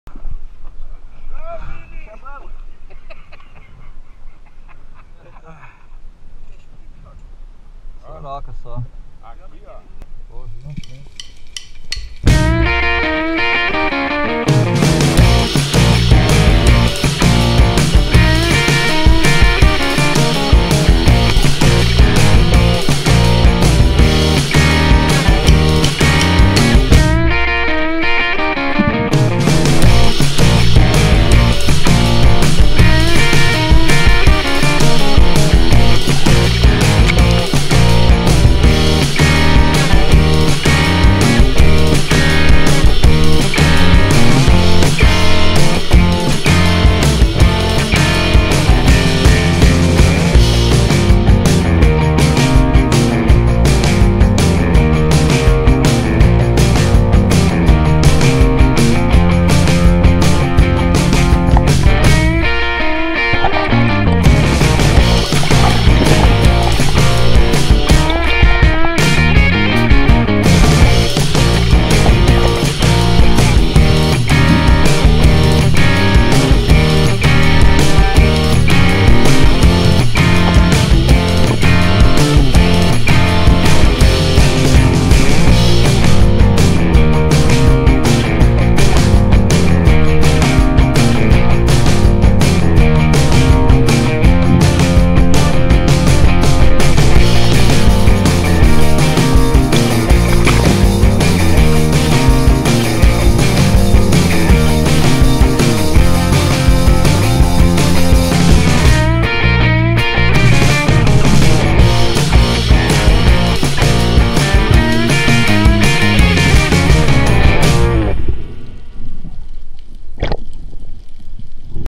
PESCA SUB - PESCARIA DE CARANHA E SOROROCA GG!
Nessa pescaria saiu caranha e sororoca GG, confira!

Previsão para a pesca, acesse